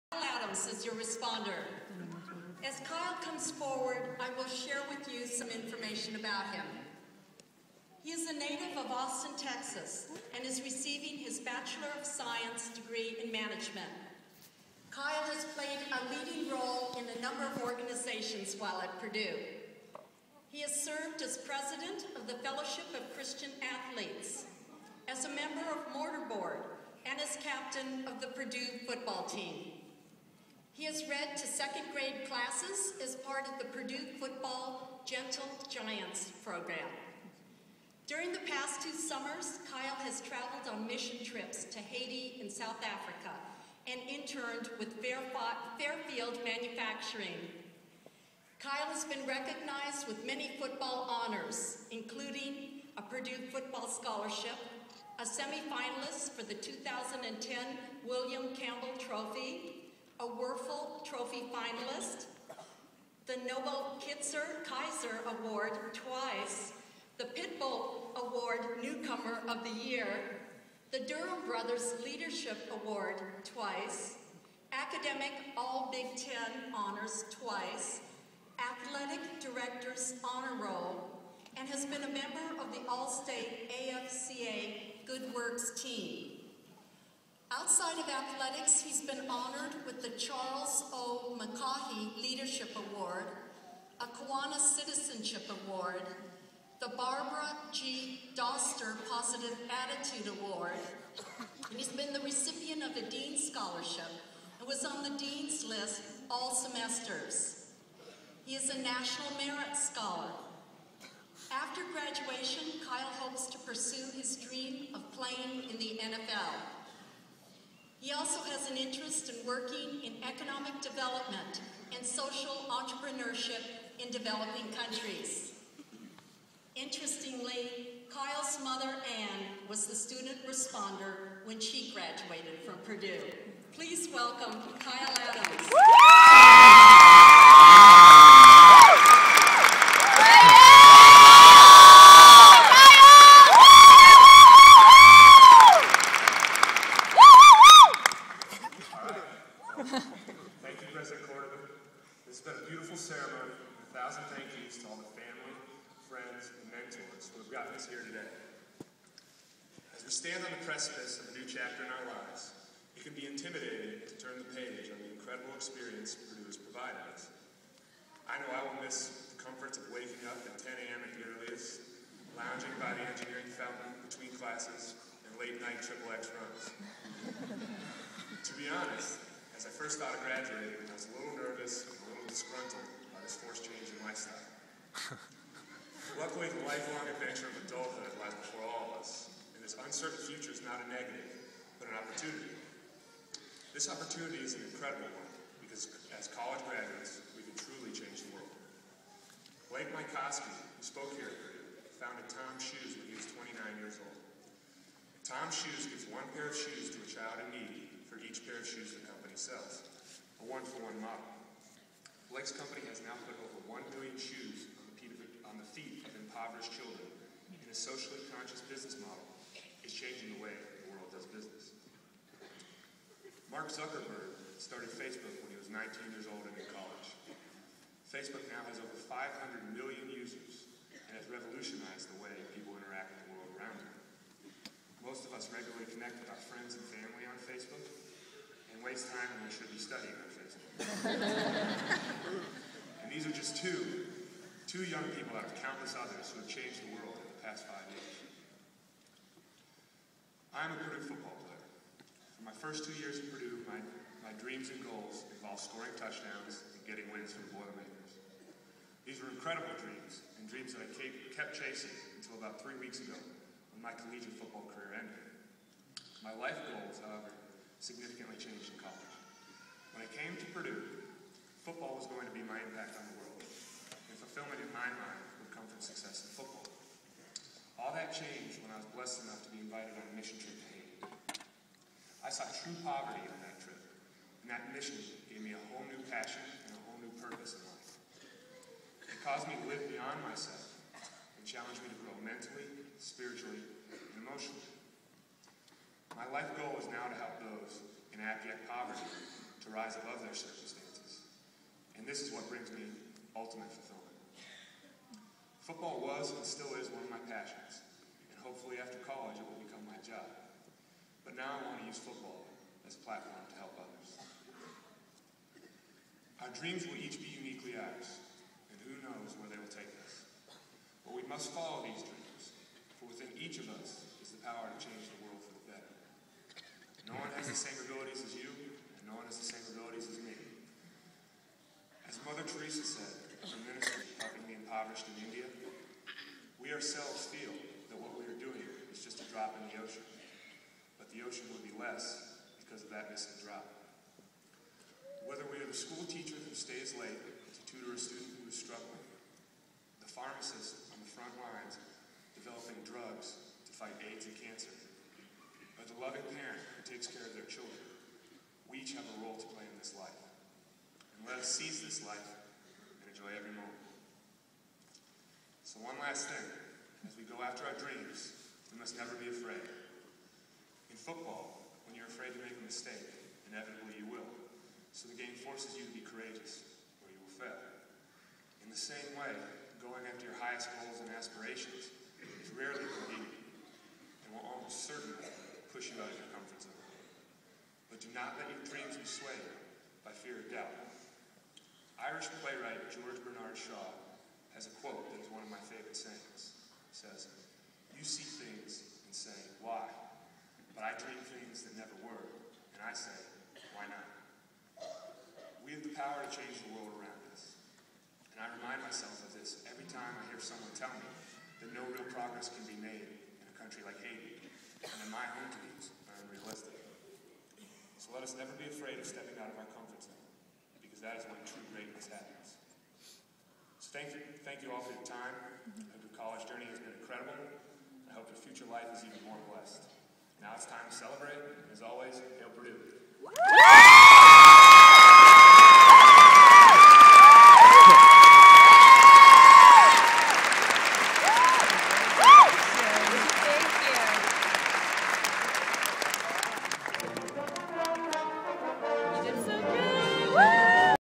Kyle Adams Purdue Commencement Speech.MOV
Kyle Adams was the "student responder" for Purdue's Graduation Ceremony in West Lafayette, IN on Sunday, December 19 at 9:30 AM.